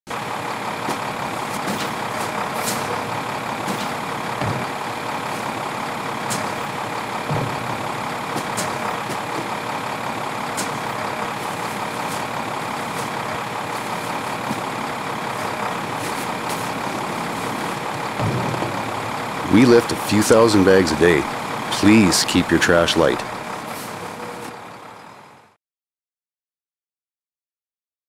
Collector Safety - Keep it Light
We lift a few thousand bags a day
Please keep your trash light

Each filled garbage bag or can should be lighter than 20 kg (44 lbs). You should be able to carry each bag or can with one hand.